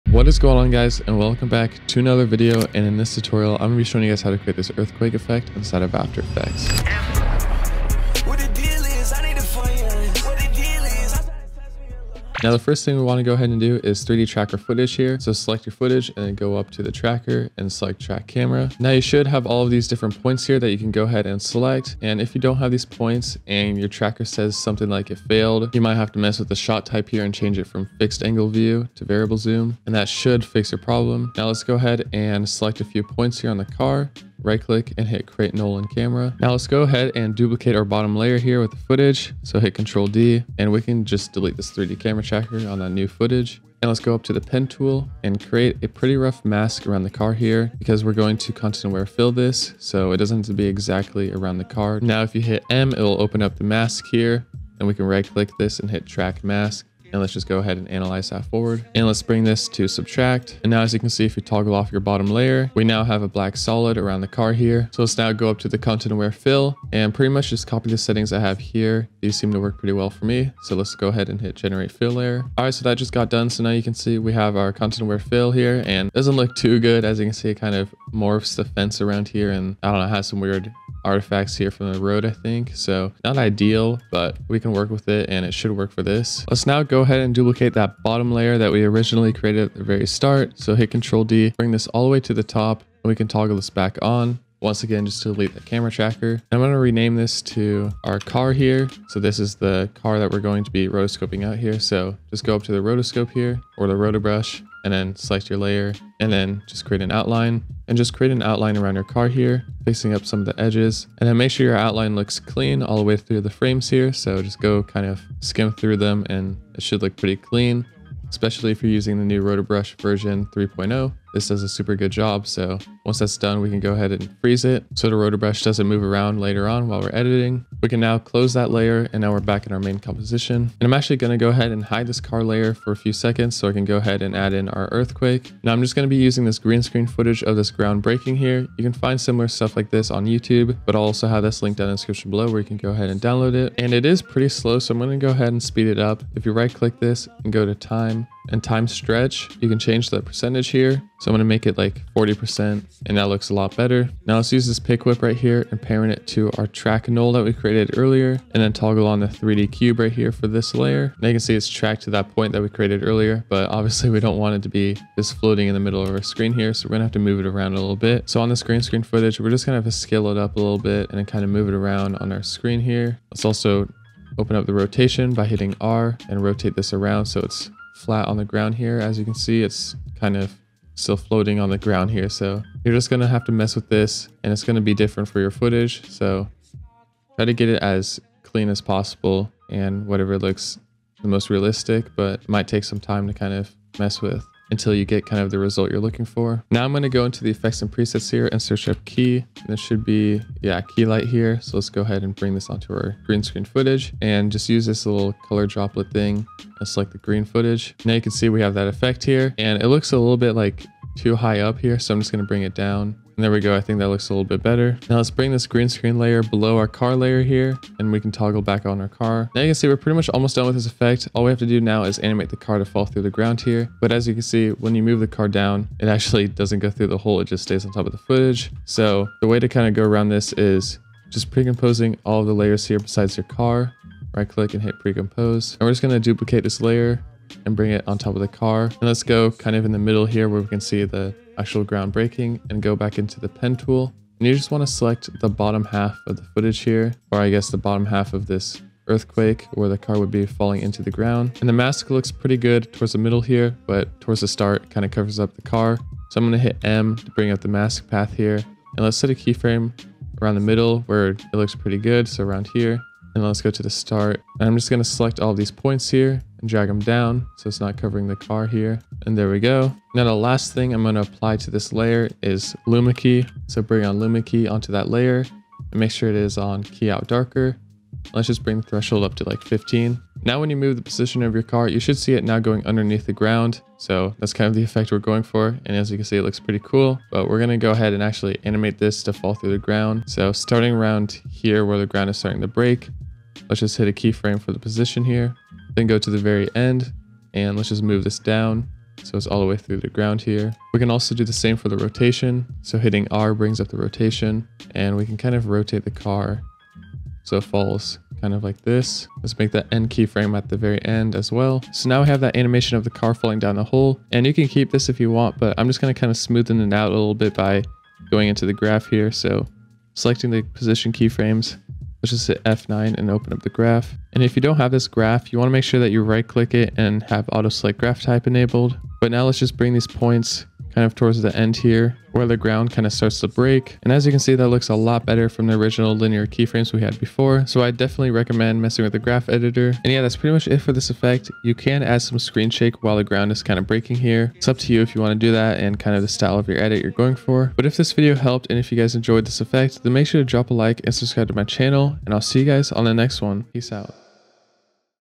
Insane Earthquake Effect - After Effects Tutorial
In this video, I go over how to create this ground cracking effect inside Adobe After Effects. Let me know what effects you want to see next in the comments below!

Car footage used in this video: @WoyshnisMedia